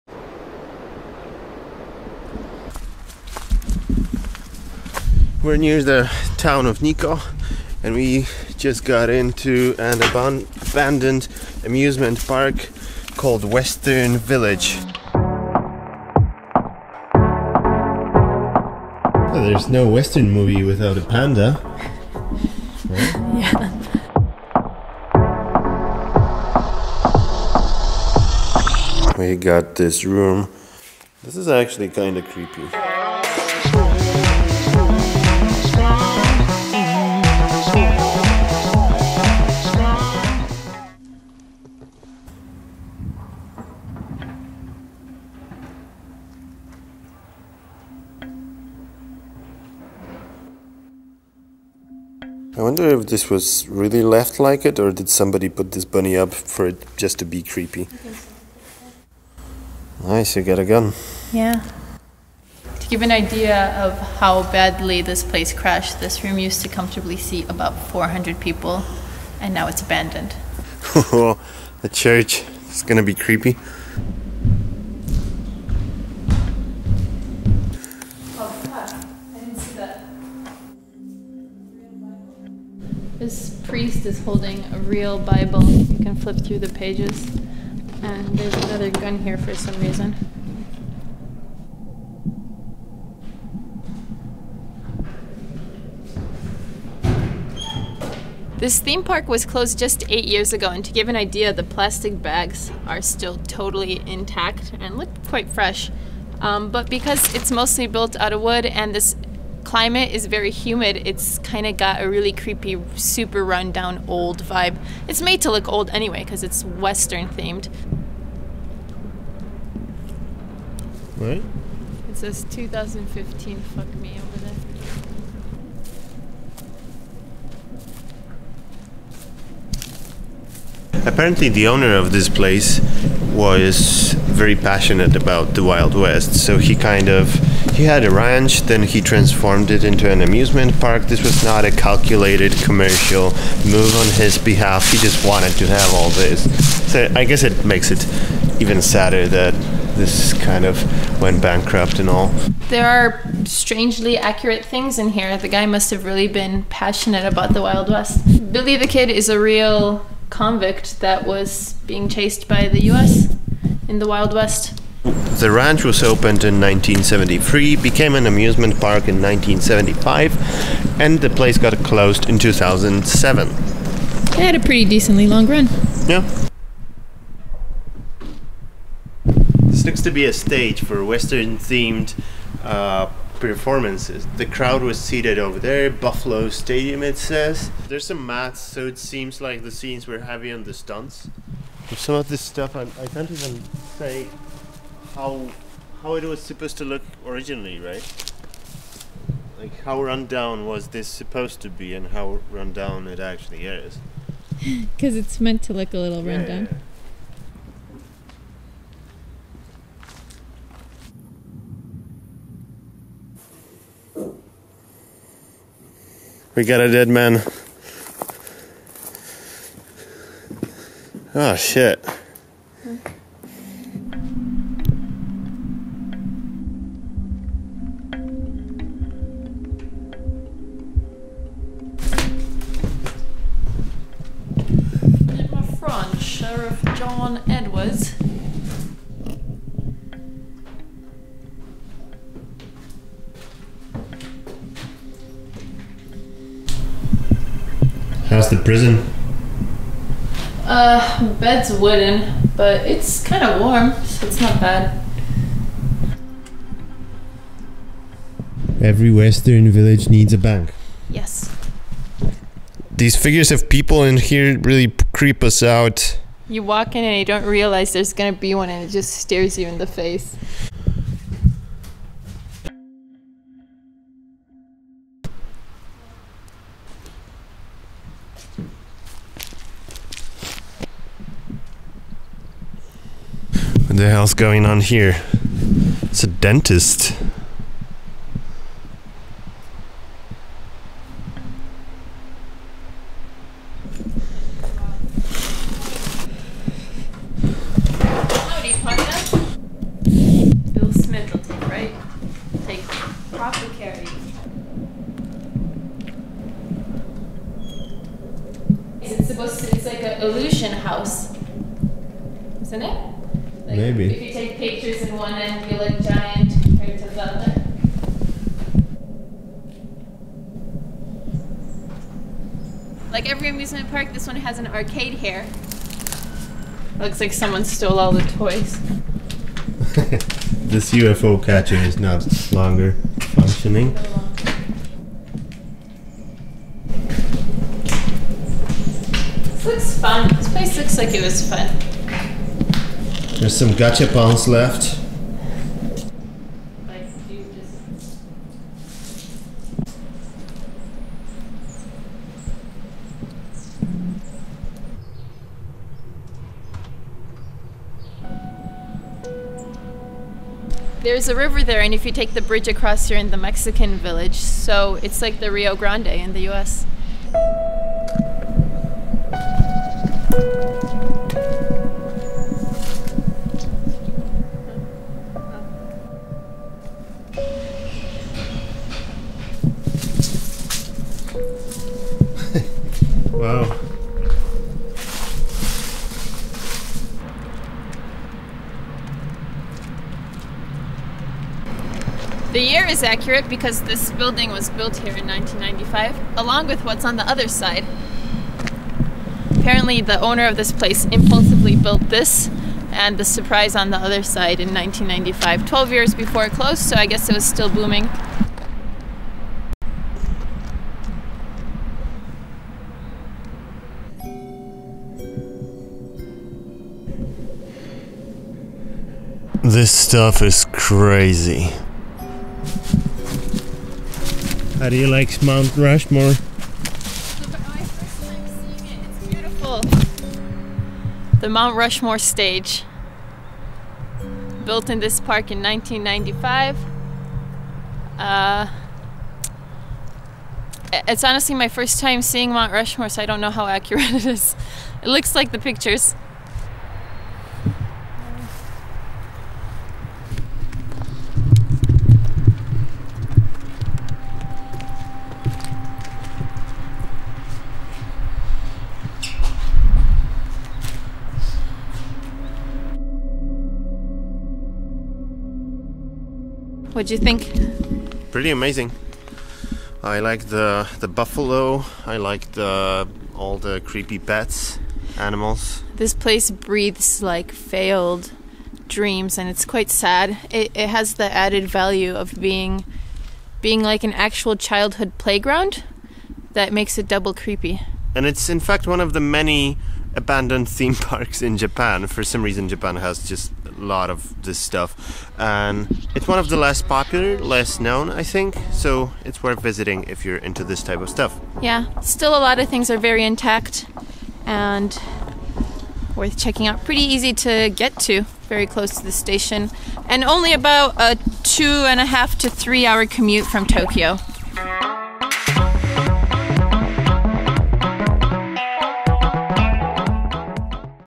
Japan's ABANDONED Wild West
Kris's Snapchat► kgonciarz
Kasia's Snapchat► kasiasia
If Westworld were in Japan and abandoned. Western Village is an abandoned amusement park near Nikko, Tochigi prefecture, Japan. It was opened in 1975 and closed in 2007. Japan thought of Westworld before it was cool.